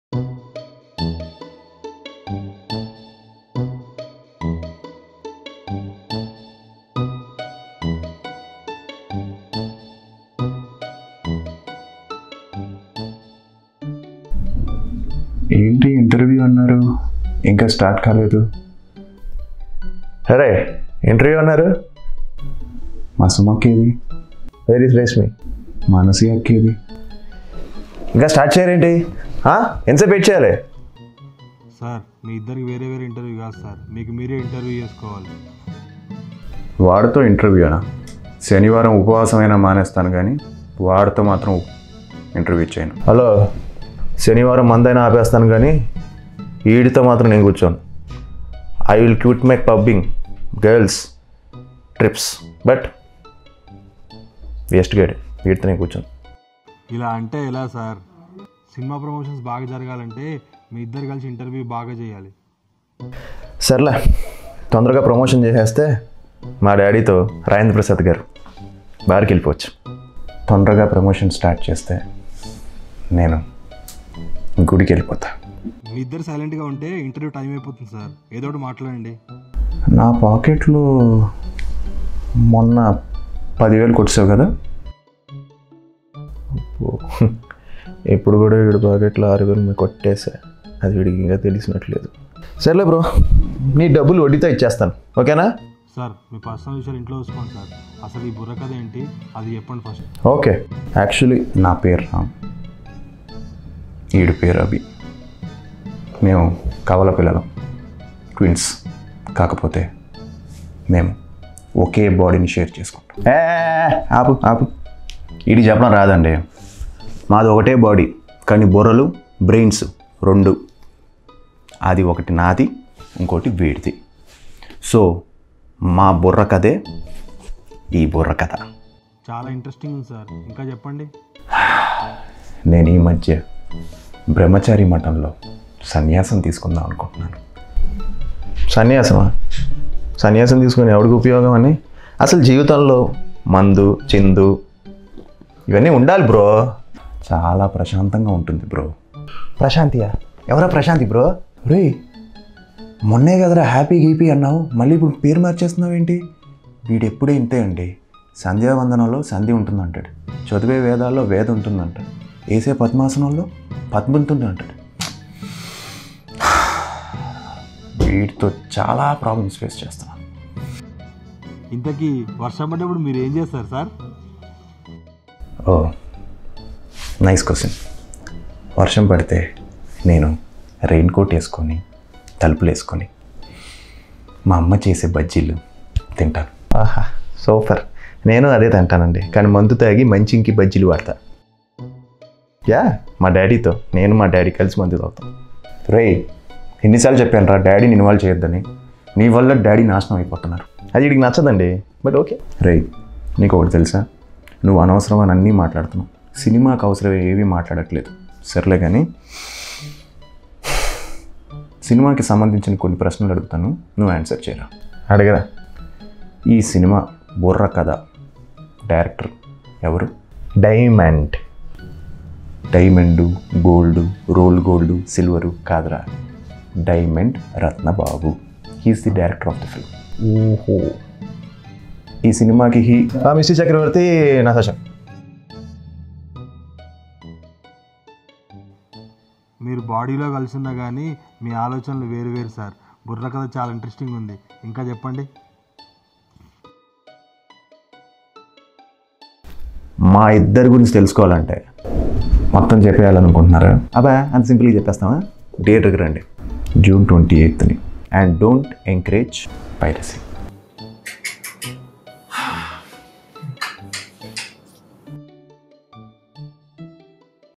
Aadi Interviewing Aadi About The Movie Burrakatha | Aadi Sai Kumar
Watch Aadi Interviewing Aadi About The Movie Burrakatha